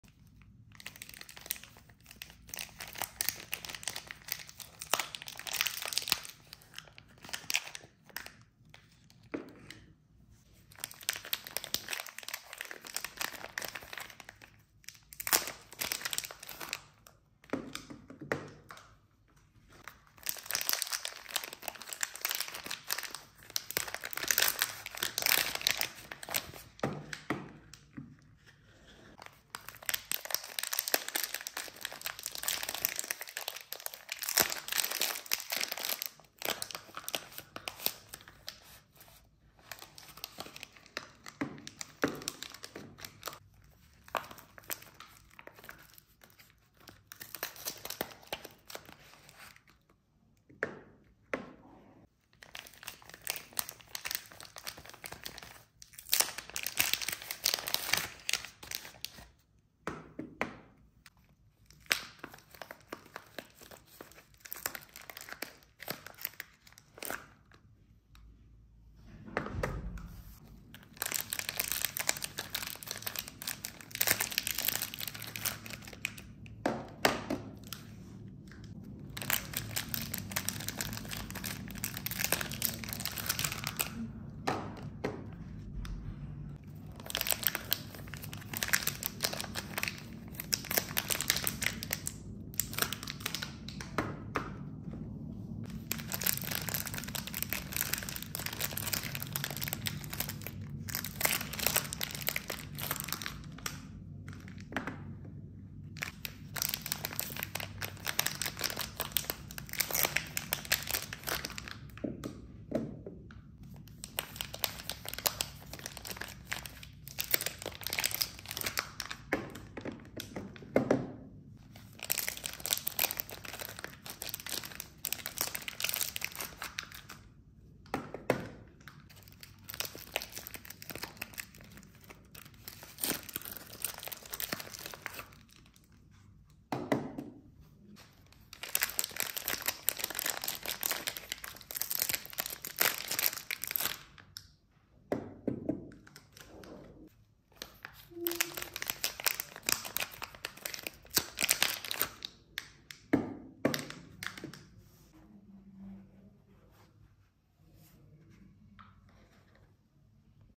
ASMR soap opening Haul no talking no music | Leisurely unpacking soap | Relaxing Soap ASMR
Hello guys and welcome to the "ASMR Venture" channel! Get ready for another portion of ASMR soap opening/unboxing/unwrapping relaxing sounds with no talking.  Hope you enjoy our oddly satisfying video.